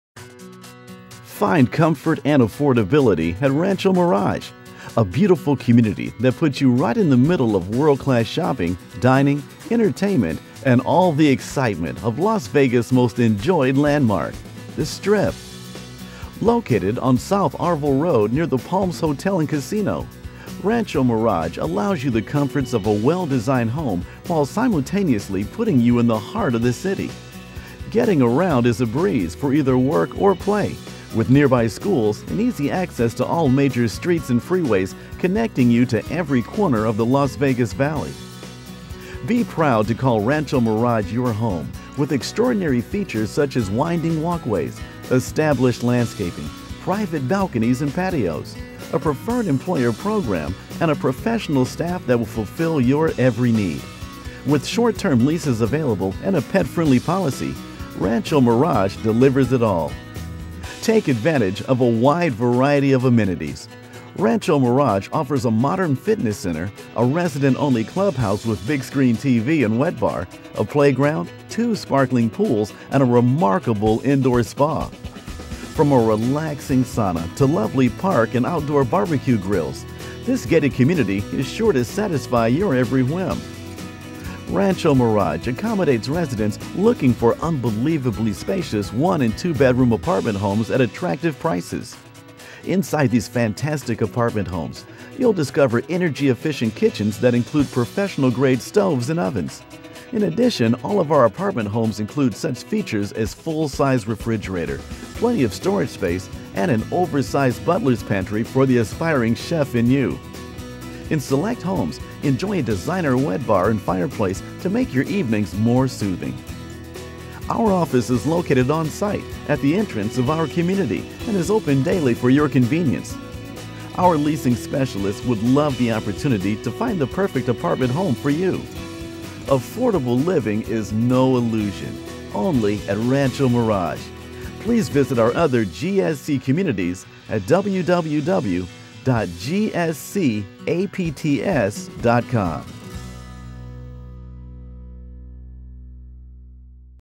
ForRent.com Rancho Mirage Apartments in Las Vegas, NV
Rancho Mirage Apartment Homes offers spacious one and two bedroom apartments featuring two pools and an indoor spa, a club house with a fitness center, energy-efficient kitchens with butler's pantry, and private patios and balconies with storage. Rancho Mirage demonstrates high standards and sincere concerns for the quality of life for their residents. Our lush landscaping and spacious apartment homes makes Rancho Mirage a lovely place to call home. View these apartments for rent online at ForRent.com